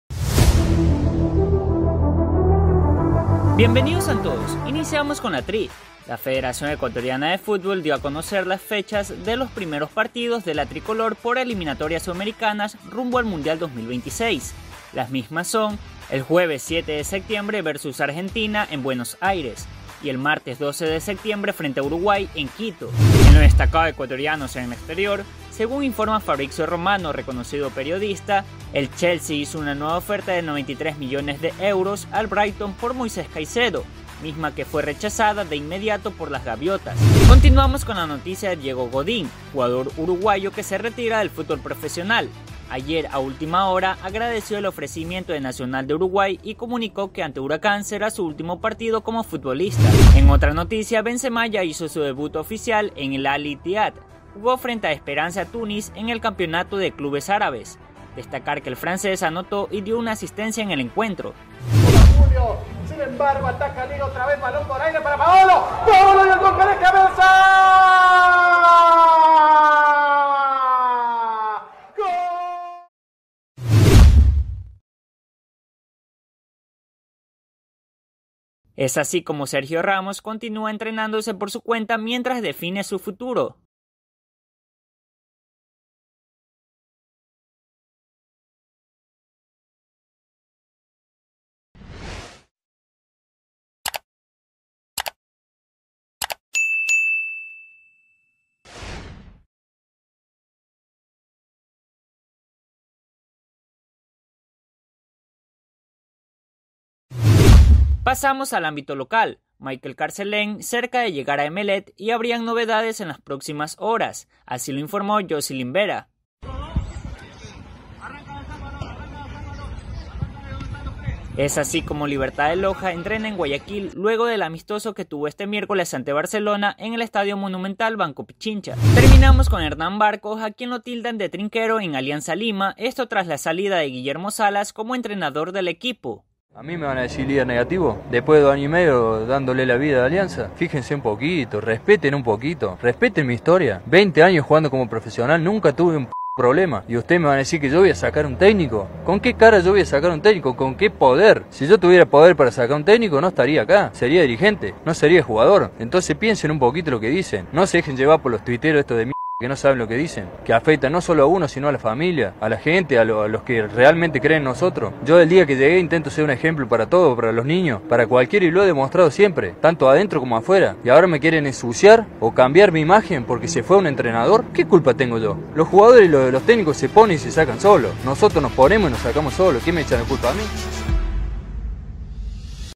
Chelsea y su oferta de €93M al Brighton por Moi | FEF: Tri y fechas de Eliminatorias | Gol de Paolo
Entérate de lo destacado de hoy.